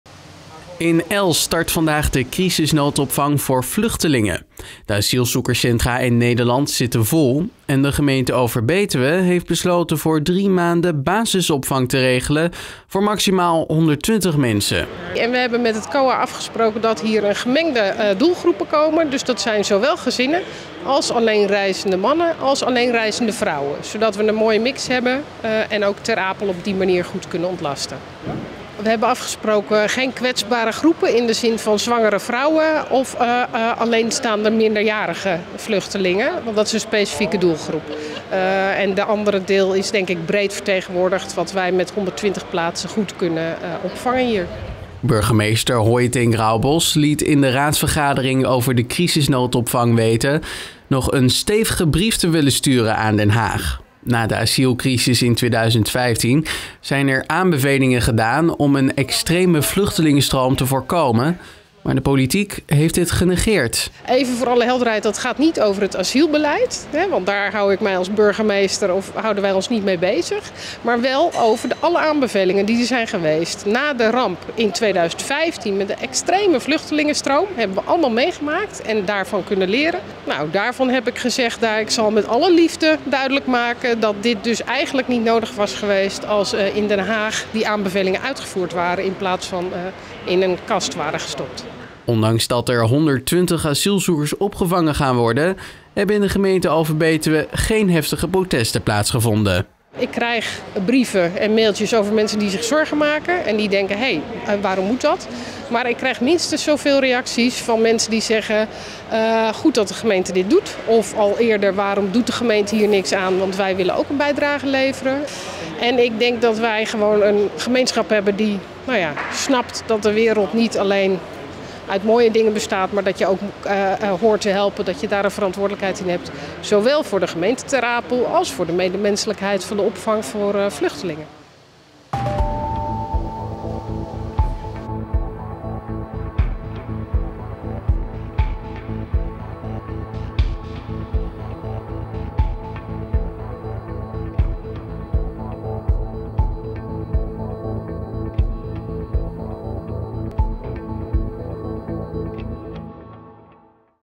Eerste asielzoekers arriveren dinsdag in Elst || RN7 REGIONIEUWS
ELST - De eerste vijftig asielzoekers komen dinsdag aan bij de crisisnoodopvang op bedrijventerrein de Aan in Elst. De gemeenteraad van Overbetuwe stemde 27 september unaniem in met de opvang om zo Ter Apel te ontlasten. Er zijn 38 units geplaatst waar plaats is voor maximaal 120 mensen. Lees verder op

Meer nieuws uit de gemeenten Beuningen, Druten, Nijmegen, Overbetuwe en Wijchen? Kijk op